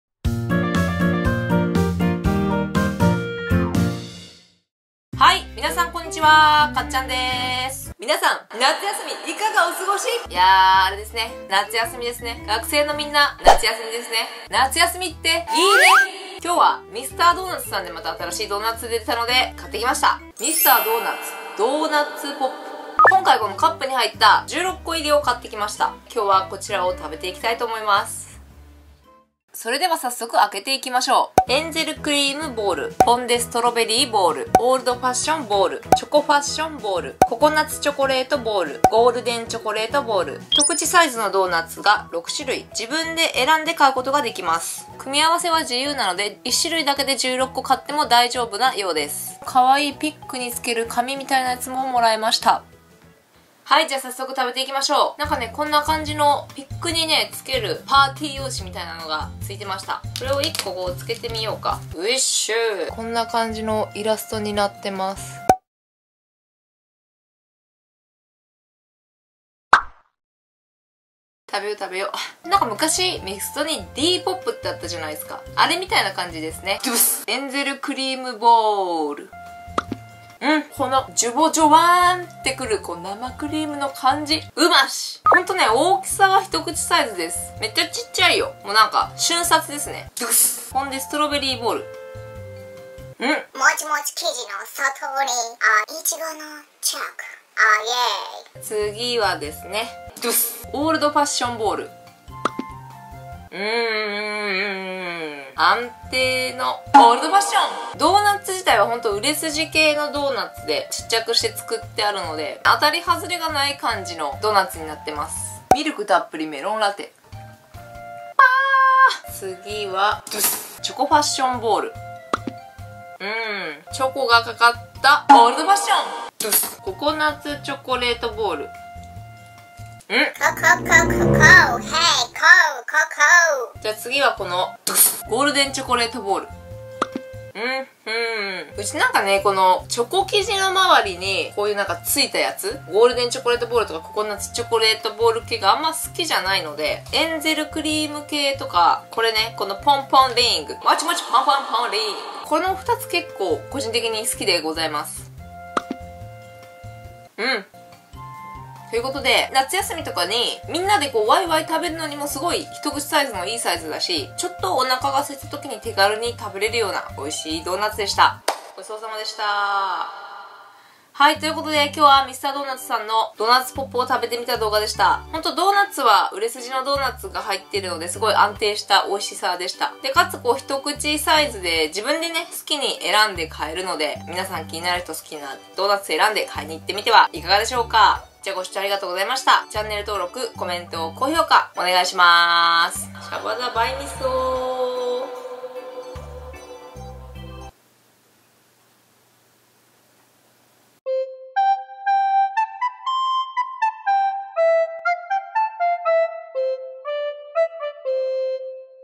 【ミスド新作】「ドーナッツポップ」全６種類を食す！【ミスタードーナツ】
ミスタードーナツさんから、一口サイズの自由に組み合わせられる手軽なドーナッツ「ドーナッツポップ」が新しく発売されていたので食べてみました！